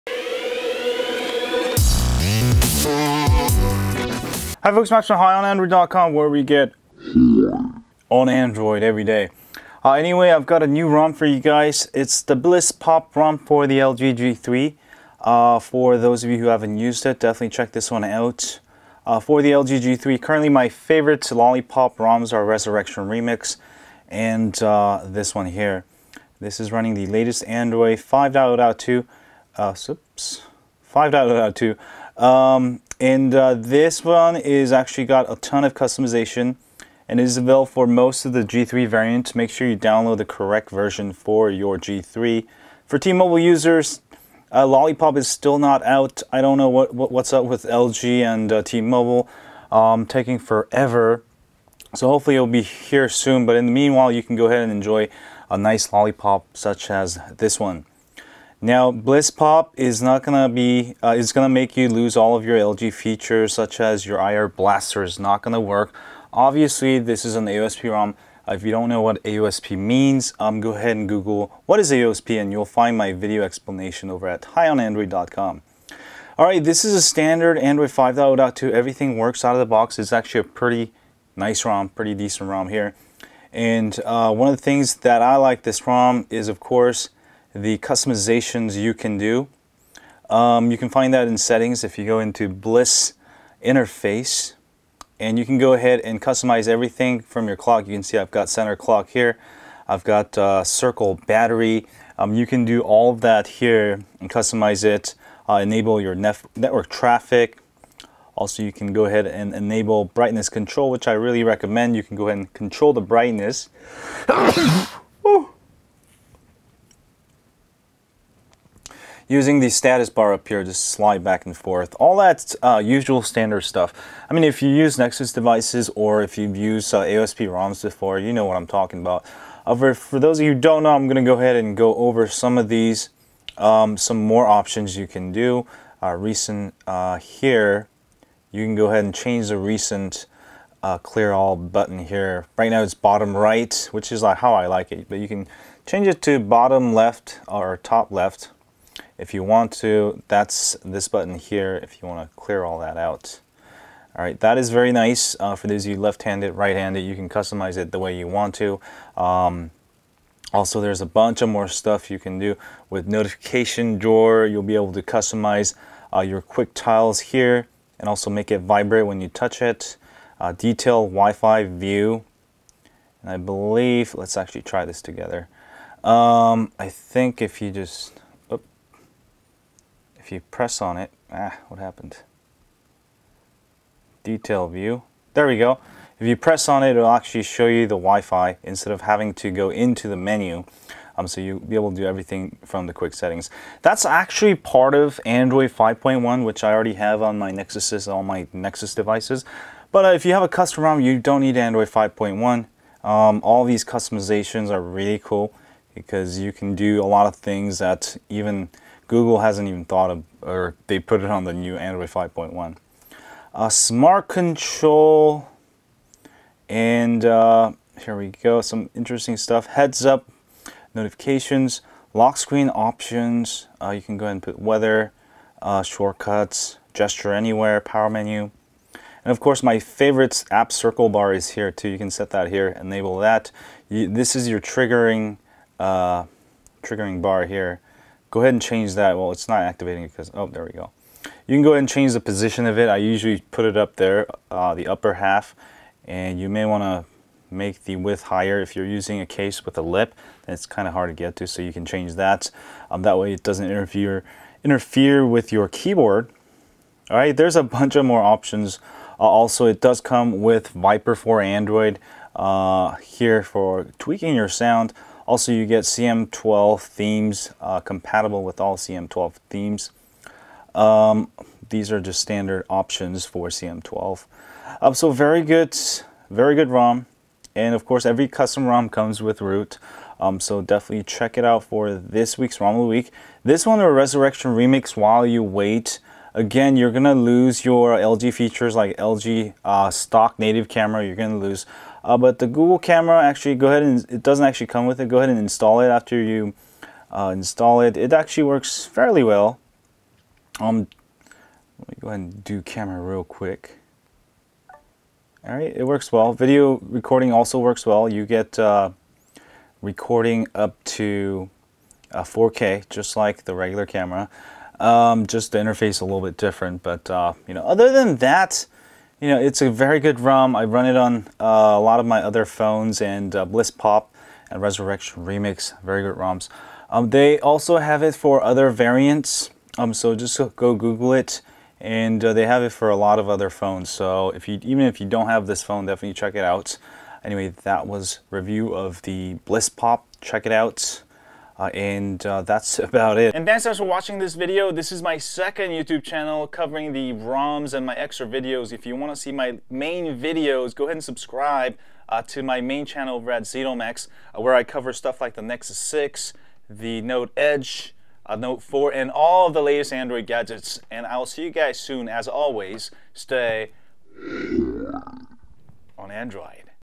BlissPop ROM for LG G3! [Lollipop/Android 5.0.2][4K]
Here's a quick overview of BlissPop ROM for LG G3 Android smartphone users.  Update your phone with Lollipop Android 5.0.2!

Camera Used for this video:
Panasonic GH4 + Sigma 18-35mm